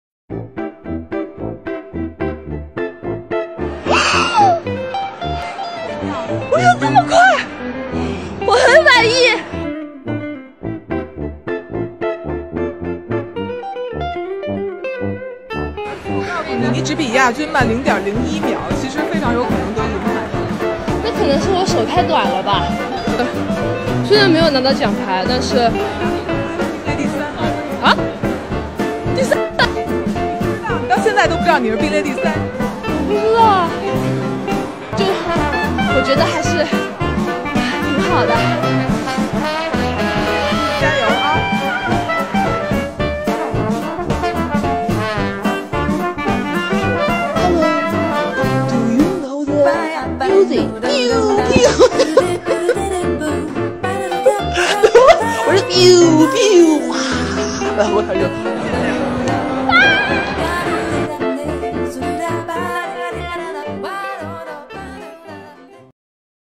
China's Fu Yuanhui: The Happiest Olympian?
The world is cemented with wars & tragedies, then there are people like Fu who make us realize how beautiful life is.